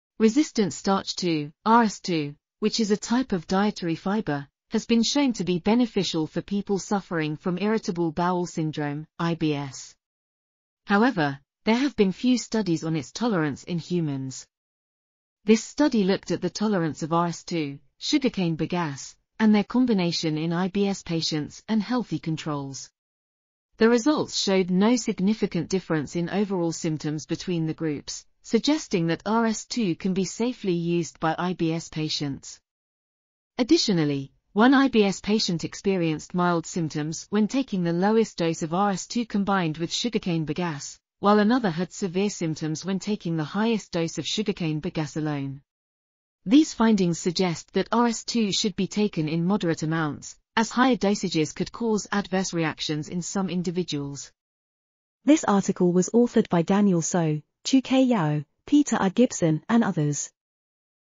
Evaluating tolerability of resistant starch 2, alone and in combination with minimall... | RTCL.TV
### Keywords ###

### Article Attribution ###
Title: Evaluating tolerability of resistant starch 2, alone and in combination with minimally fermented fibre for patients with irritable bowel syndrome: a pilot randomised controlled cross-over trial
Authors: Daniel So, Chu K. Yao, Peter R. Gibson ,and Jane G. Muir
Publisher: Cambridge University Press
DOI: 10.1017/jns.2022.9

### Video Timestamps ###
0:00:00 - Summary
0:00:55 - Title
0:01:01 - End